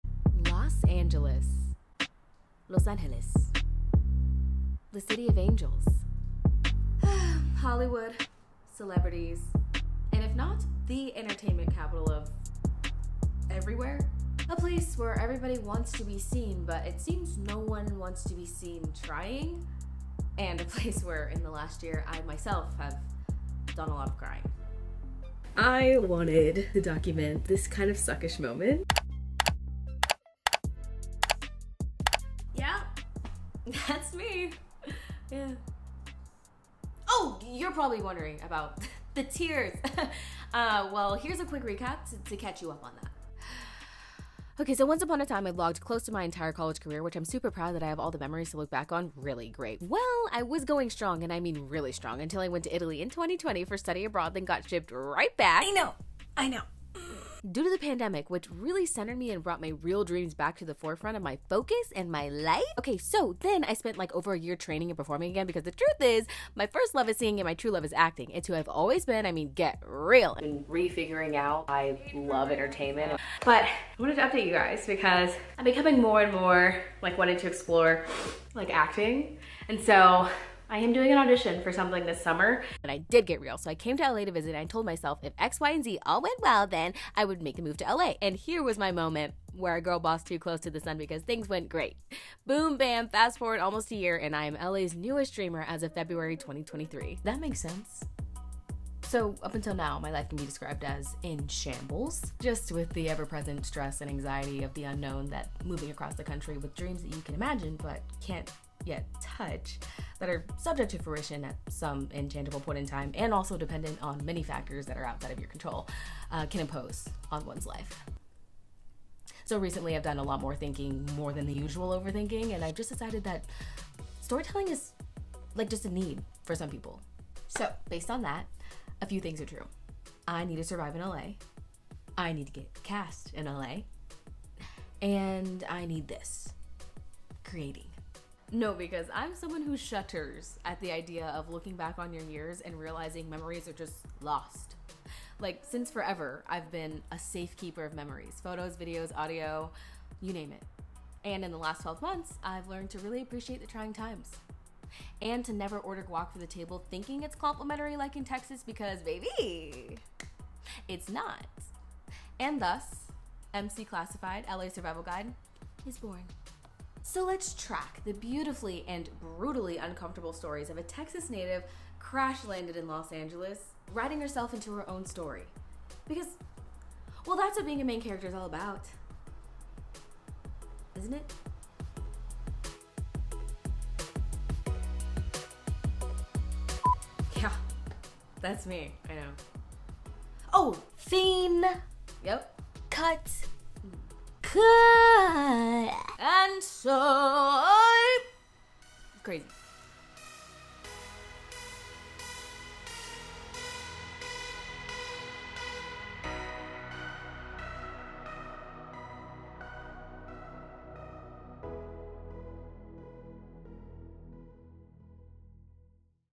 I'm not getting cast in Los Angeles so I'll do it myself
Hi again, YouTube.
Long time, no yap. Welcome to back to all my nuggets of wisdom and intrusive thoughts. & Introducing the As Told By Mereyah, the struggles of me making it in Los Angeles as a creative in a city of creatives.